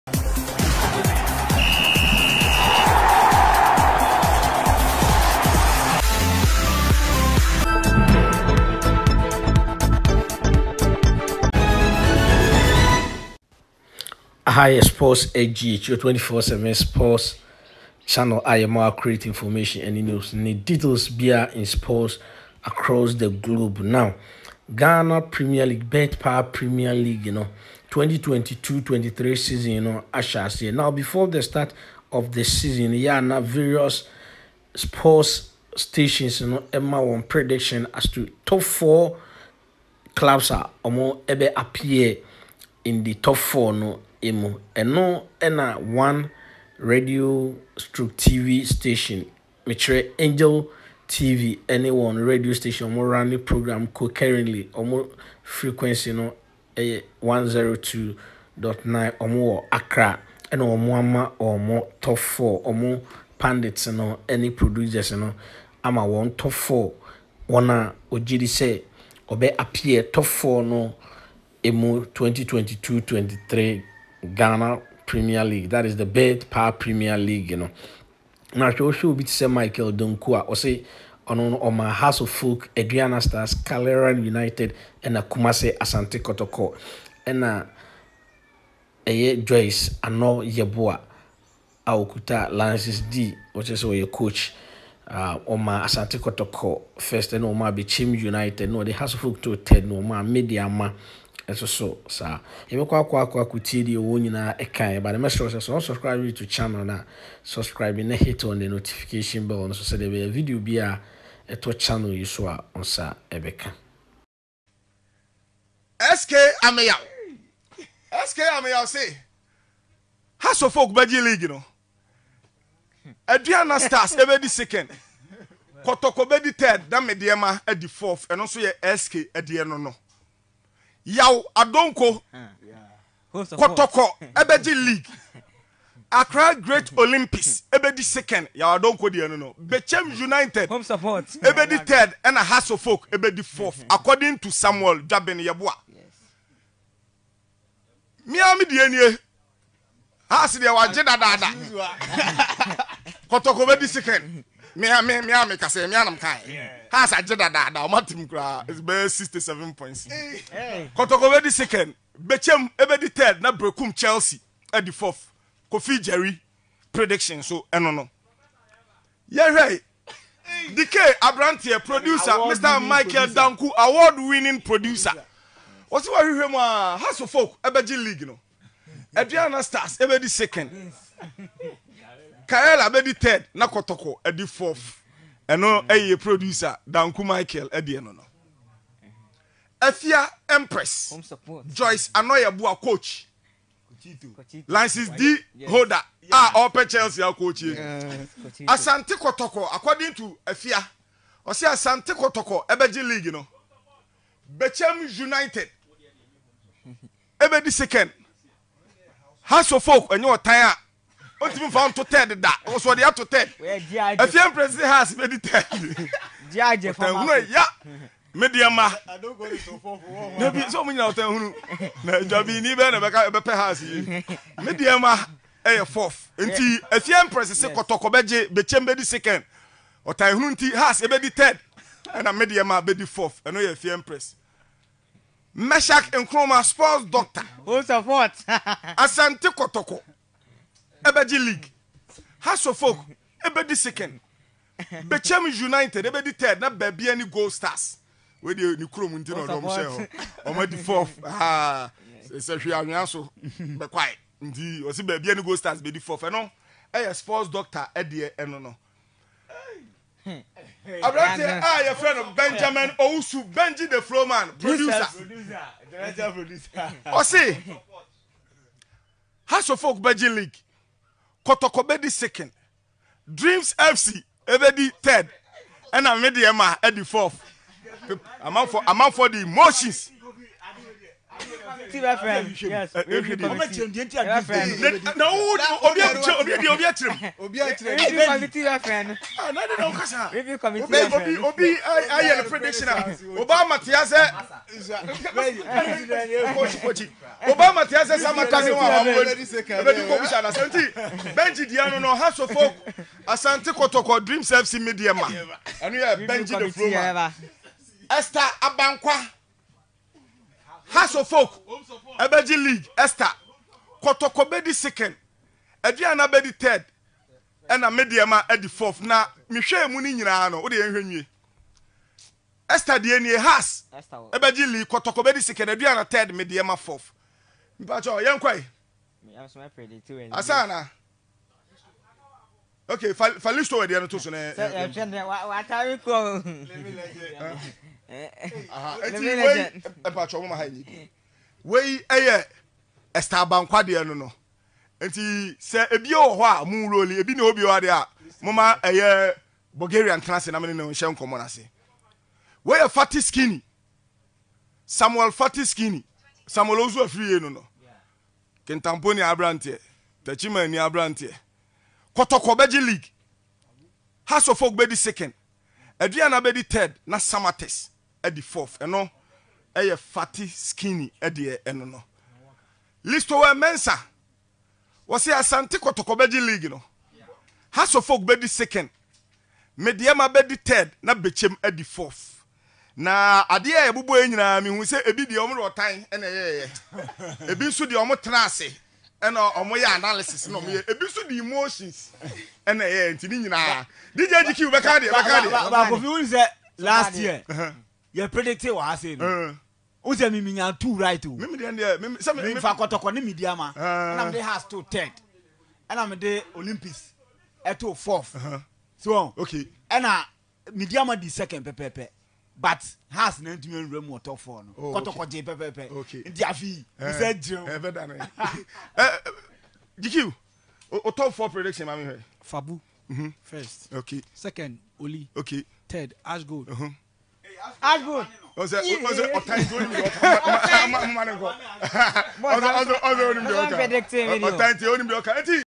Hearts of Oak // Favorite to win the 2022/2023 premier league…
Angel fm in Accra GHANA sports crew predict their top 4 clubs for 2022/2023 Ghana premier league ...

... @YAA YAA TV @TOTO SPORTS GH @PHOBIA HEADQUARTERS @Proud Phobians